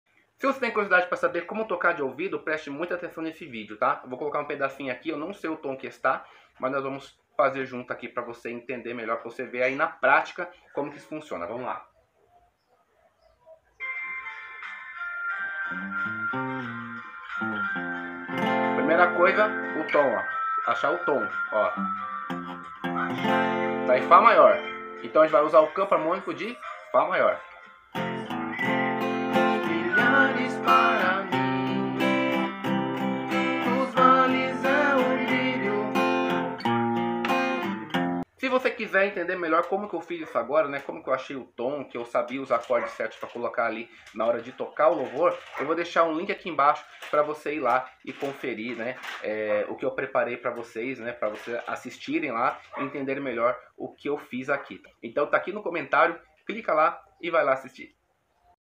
COMO TOCAR DE OUVIDO? VEJA NA PRÁTICA, EM MENOS DE 1 MINUTO, NESTE VÍDEO DEMONSTRATIVO
VOU DEMONSTRAR, NO VIOLÃO, COMO TOCAR DE OUVIDO EM UM VÍDEO DE MENOS DE 1 MINUTO.

Link para o outro vídeo